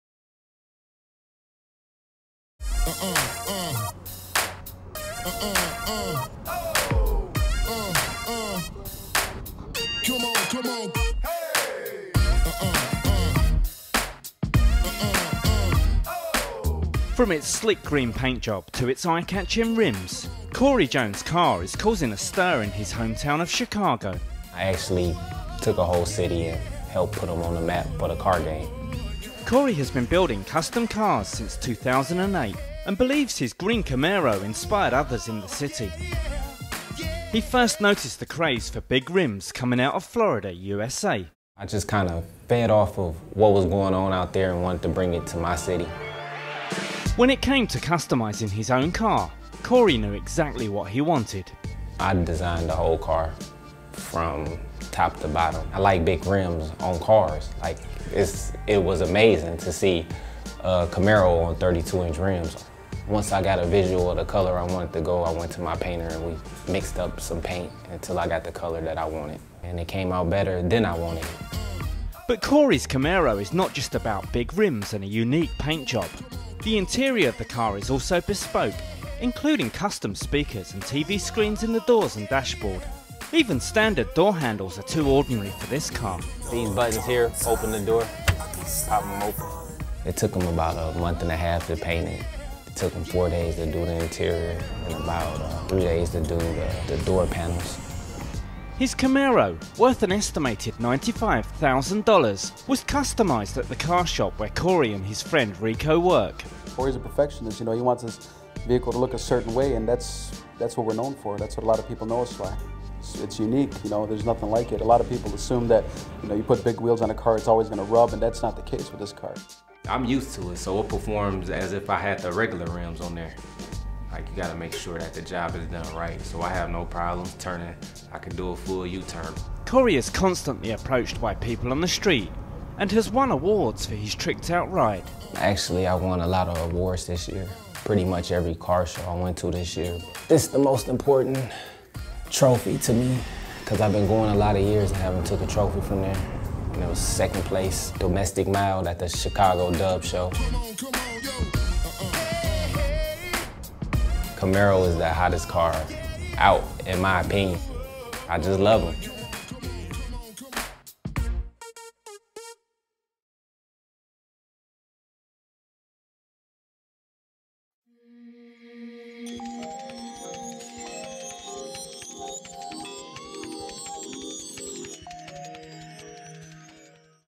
Throwback of my CAMARO ON 32'S
My first interview in my drop top CAMARO ON 32 INCH DUBS.. This put the city on the map as they came from another country to film me...make sure to follow me on Instagram CJ_ON_32S

IF YOU'RE READING THIS COMMENT " I MISS THE GREEN COLORWAY "